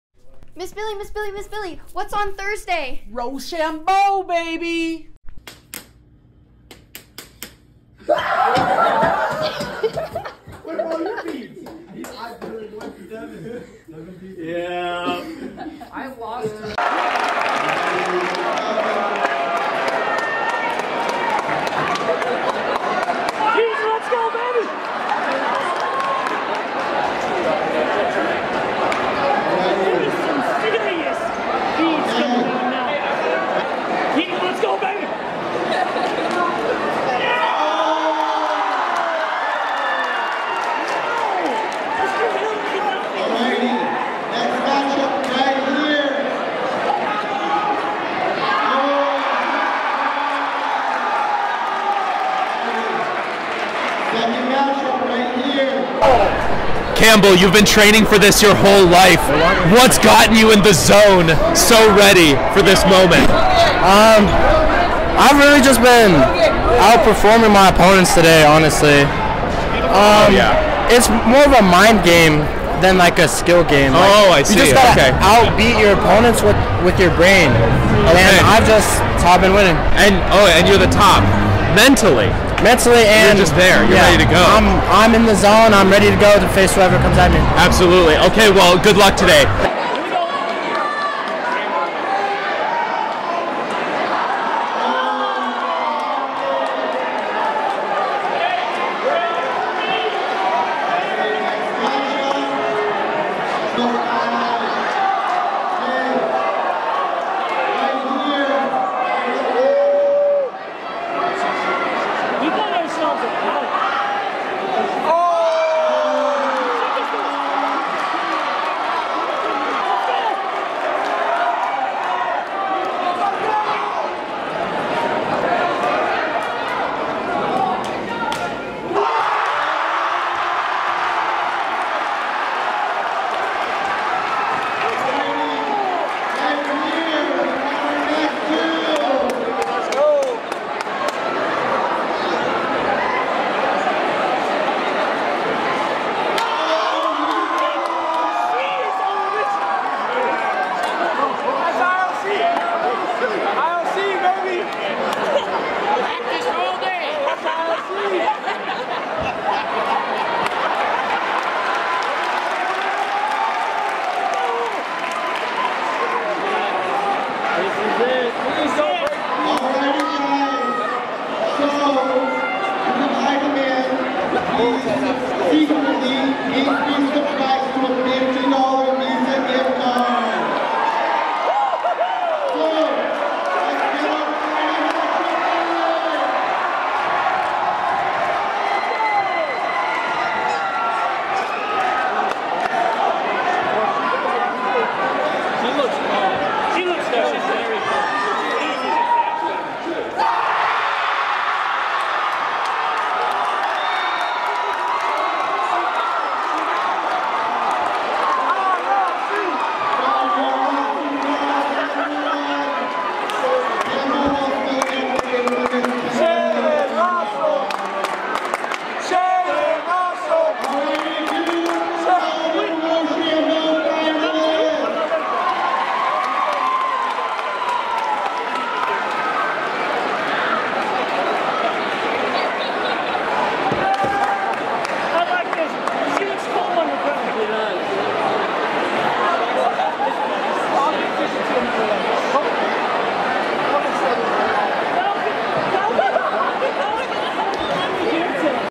Skyview High School Ro-Sham-Bo Tournament
Congratulations to Cheylynn Russell, for winning today's Ro-Sham-Bo Tournament! All students and staff received a beaded necklace when entering the school this morning. If you won a game of Ro-Sham-Bo, you get to take the losers beads.